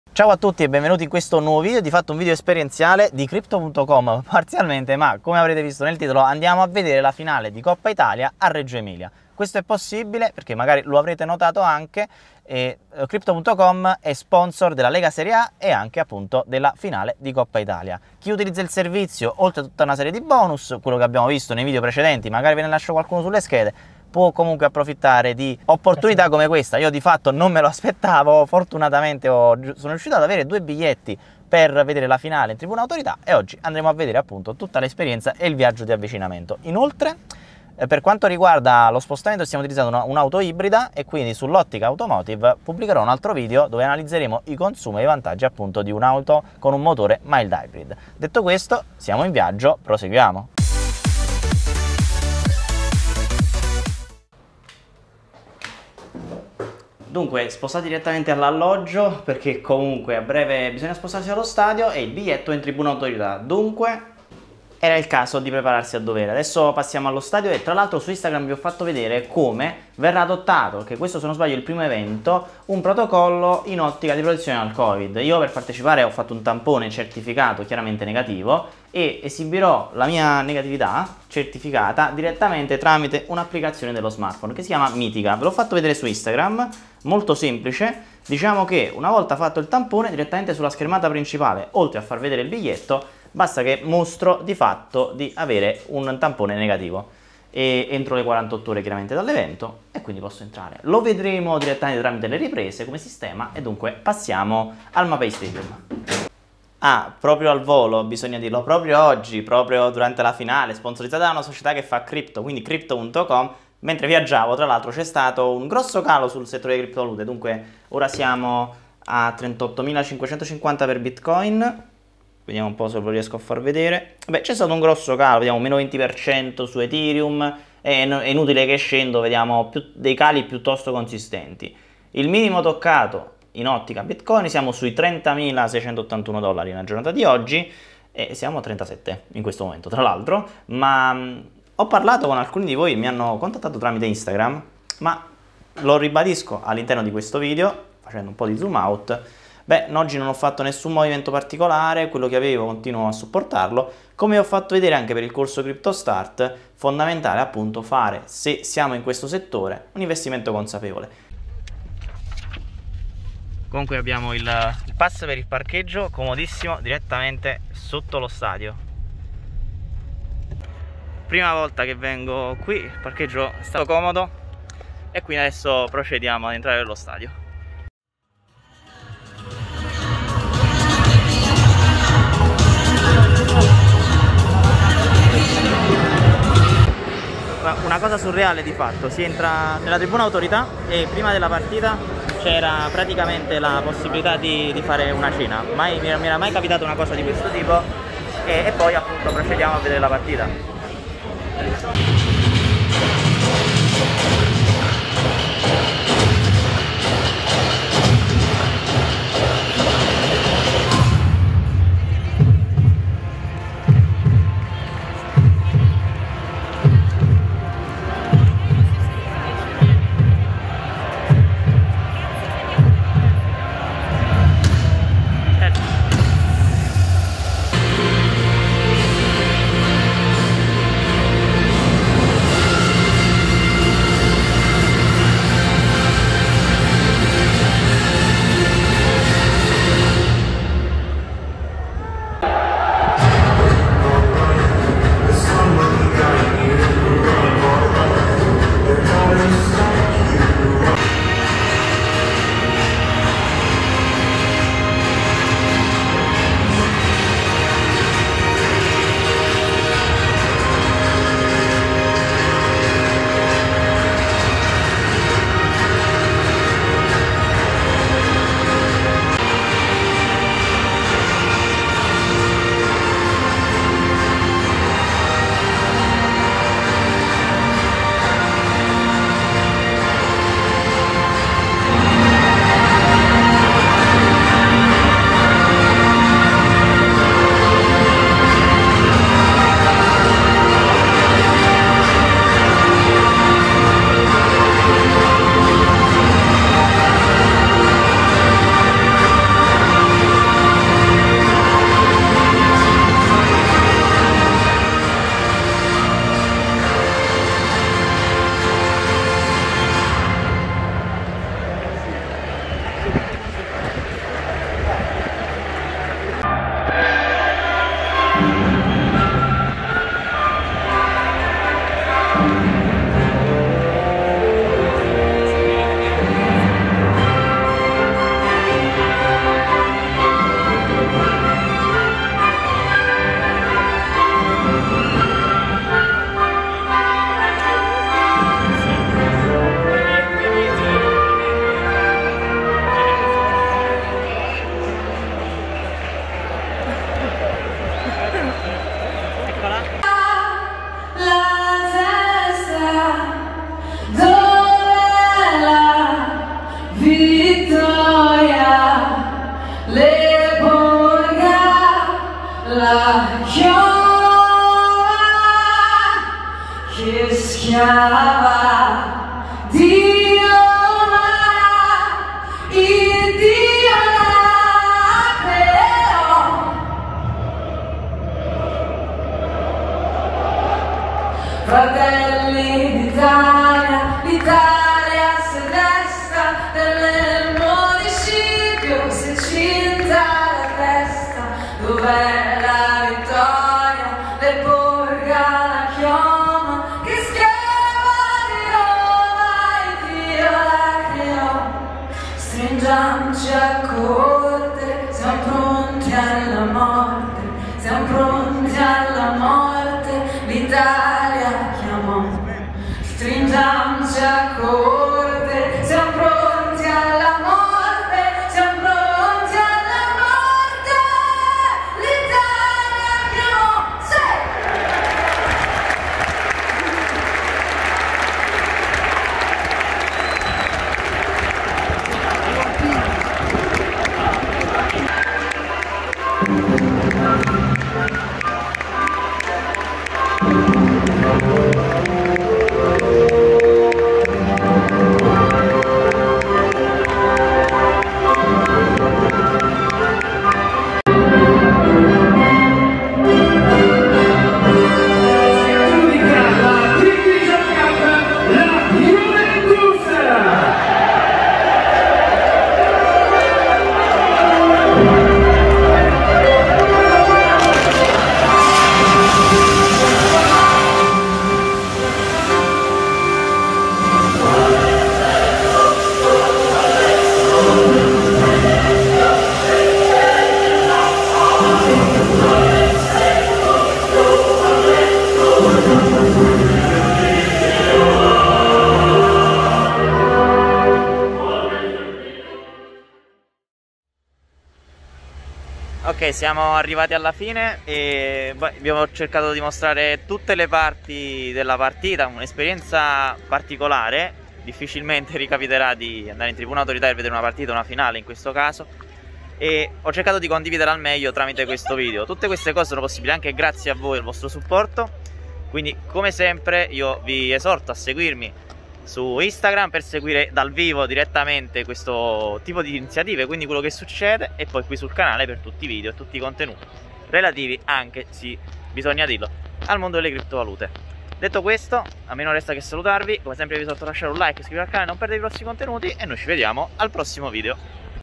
Finale di Coppa Italia Atalanta Juventus 1-2 in Tribuna Autorità con Crypto.com - Esperienza Unica
Crypto.com come sponsor ufficiale mi ha permesso di poter assistere in Tribuna Autorità alla finale di Coppa Italia tra Atalanta e Juventus, finita 1-2.

Invito totalmente inaspettato e che mi ha visto organizzare il tutto in qualche ora. Vediamo il viaggio e l'esperienza all'interno del Mapei Stadium di Reggio Emilia con i momenti salienti della giornata, più un update sulle criptovalute che proprio il giorno della finale hanno visto un calo consistente.

Se abbiamo investito in modo consapevole poi, poteva mai impensierire? Chiaramente no :D

➤ Link Amazon per sostenere il canale in modo gratuito, salvalo tra i tuoi preferiti

➤ Trovi qui i componenti

ATTREZZATURA VIDEO
➤ La trovi qui